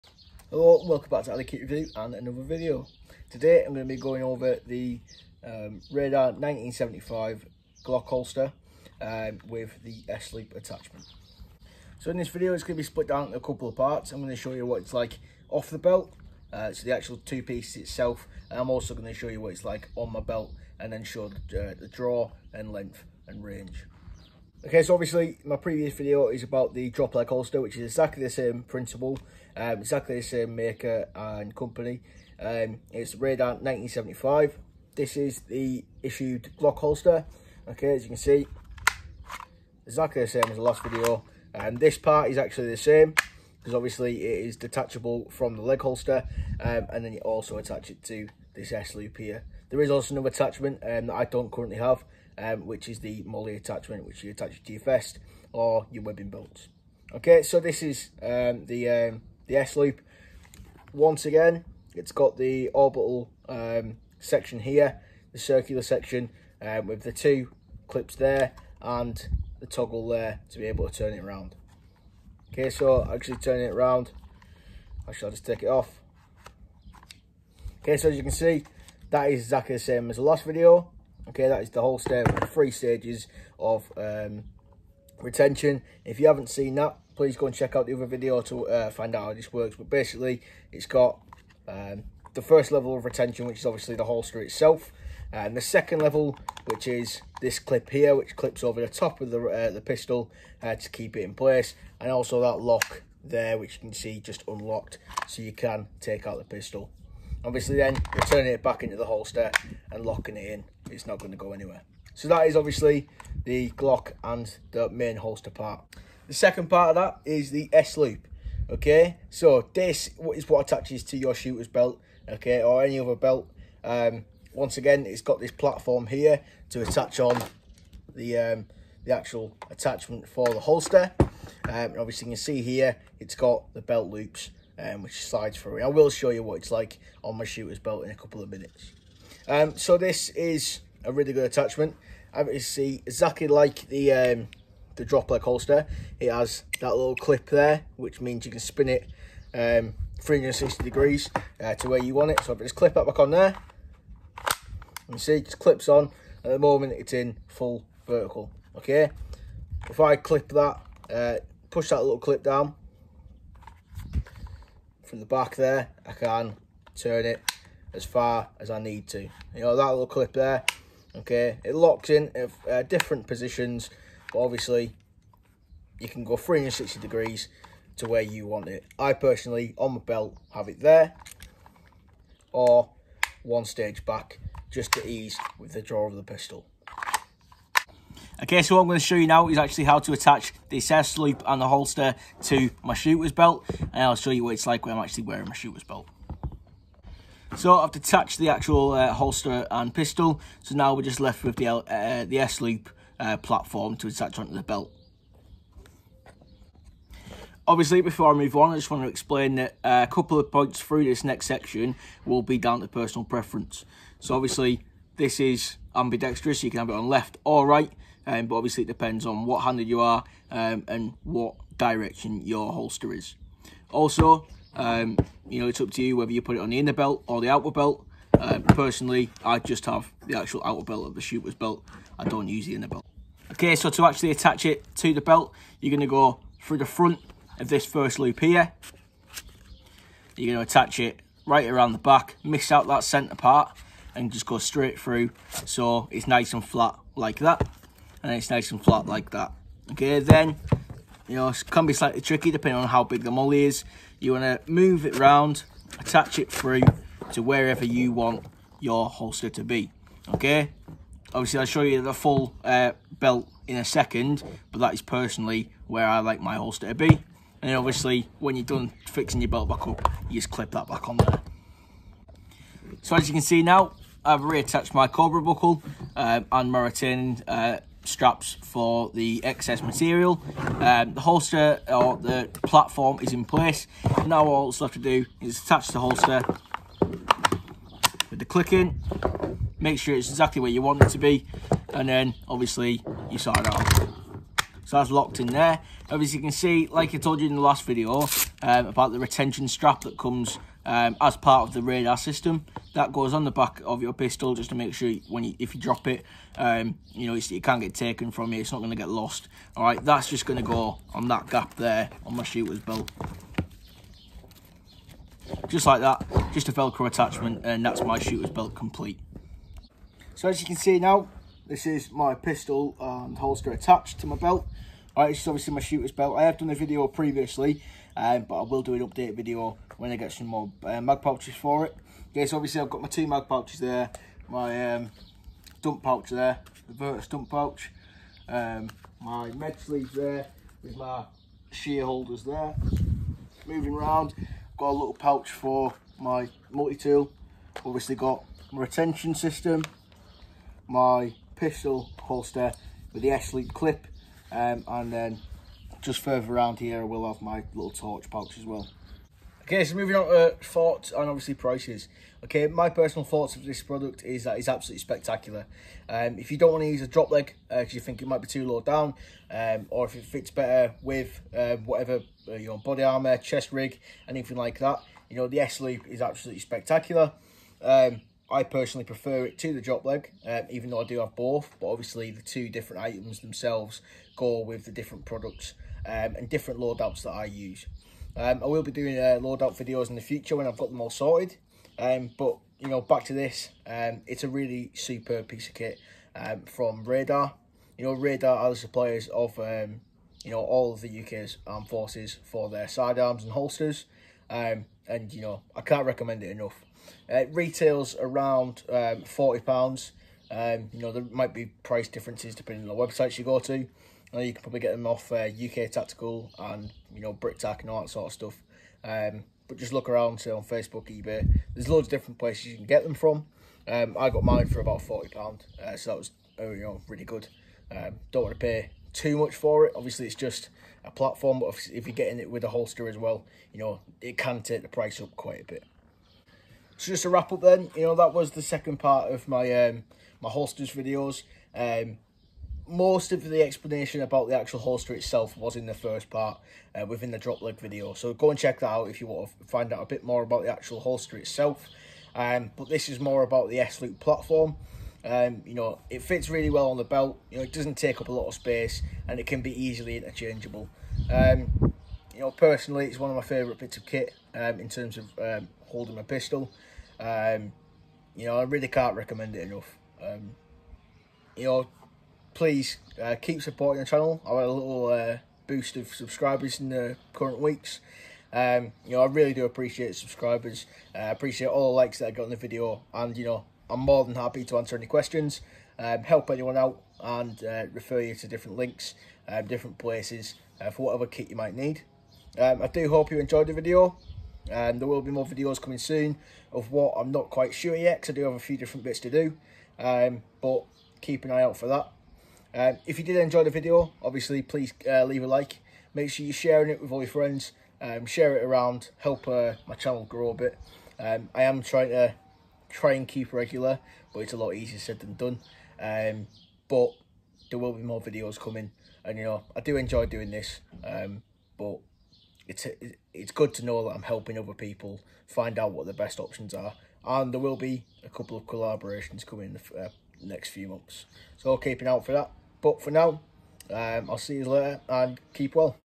Radar 1957 S loop glock holster
This is the S loop platform used by British forces to mount the glock holster onto a belt or shooters belt.

My apologies. It’s 1957 not 1975 as I said at the start of the video. Confusion